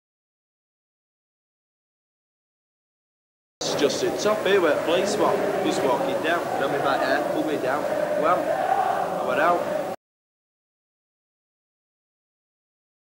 Leeds United: Young stupid Steve
Leeds United v Manchester United 1974. A kick in the eye for me.